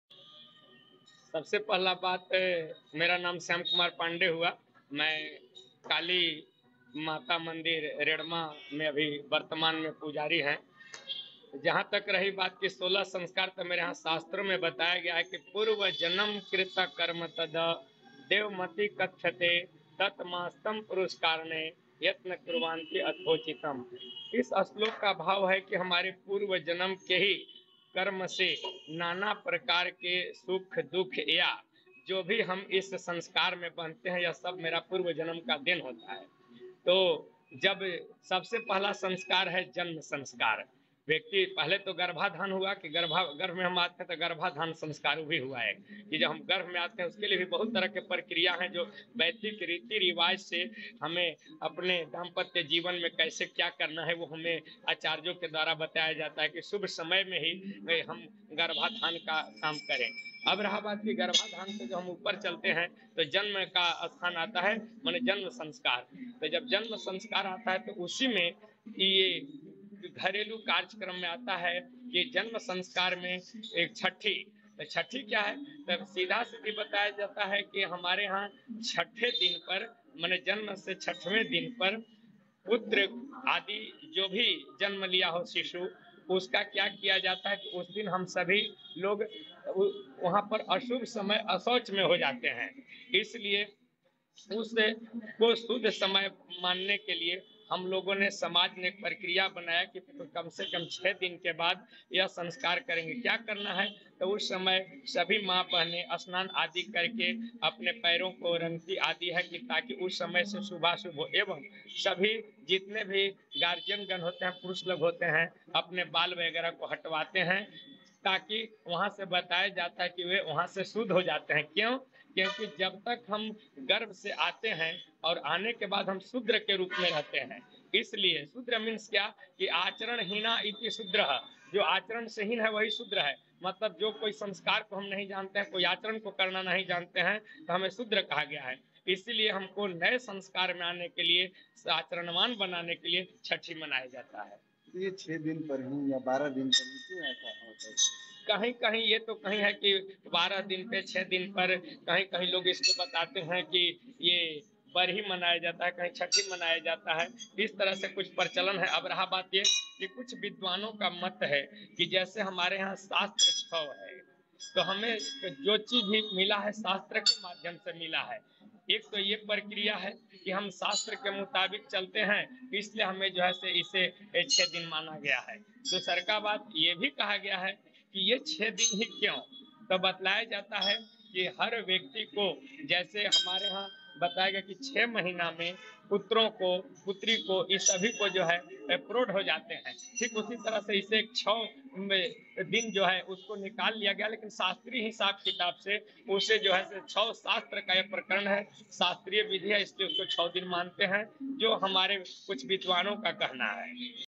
Astro Tips: शिशु जन्म के बाद क्यों मनाई जाती है छठी, जानें इसका महत्व
Priest Shyam Kumar Pandey told Local 18 that Mata Chhath is worshiped on this day. In which the women and men of the house join and celebrate like a festival. It is also believed that the festival was celebrated six days after the birth of Lord Rama. Because of which this tradition has been going on even today.

पुजारी श्याम कुमार पांडेय ने लोकल 18 से कहा कि इस दिन माता छठी की पूजा होती है. जिसमें घर के महिला पुरुष शामिल होकर उत्सव की तरह मनाते है. मान्यता ये भी है कि भगवान राम के जन्म के छः दिन बाद उत्सव मनाया गया था. जिस कारण से ये परंपरा आज भी चलती आ रही है.